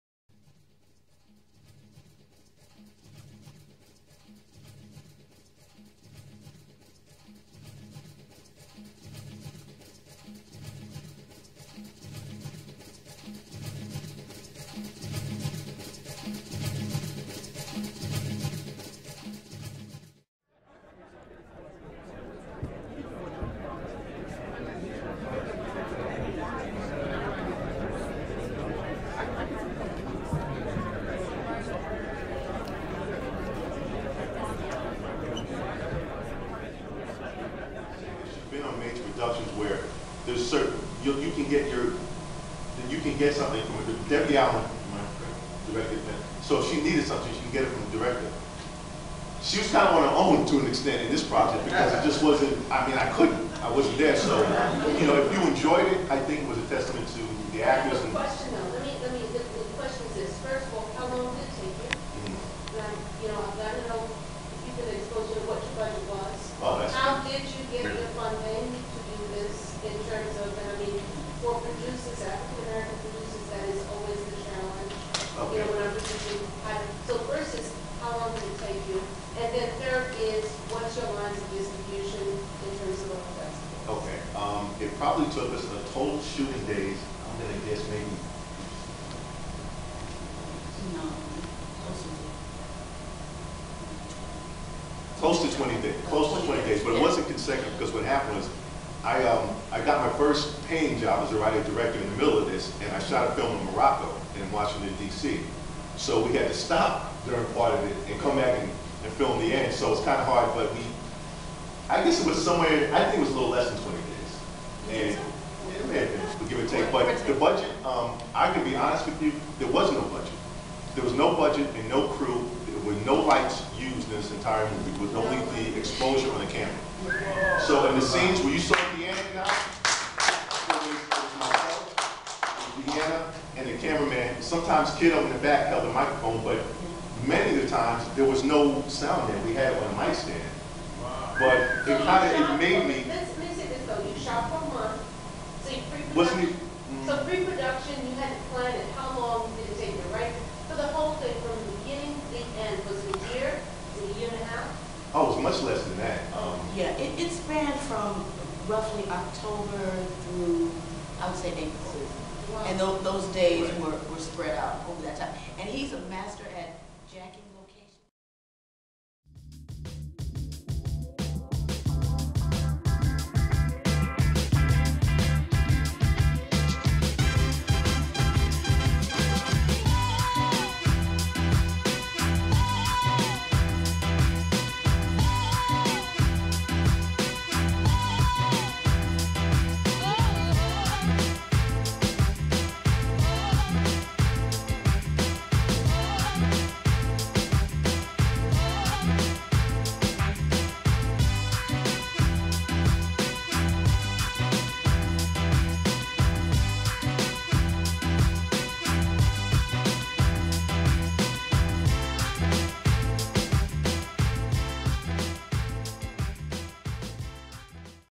Q&A - THE NEXT DAY Cast and Filmmaker, Part 2
The cast and filmmaker of THE NEXT DAY participate in a Q&A session at the 10th International Black Women's Film Festival on Saturday, July 21, 2012 at the Koret Auditorium - San Francisco Public Library on 100 Larkin Street, San Francisco.

In collaboration with the San Francisco Public Library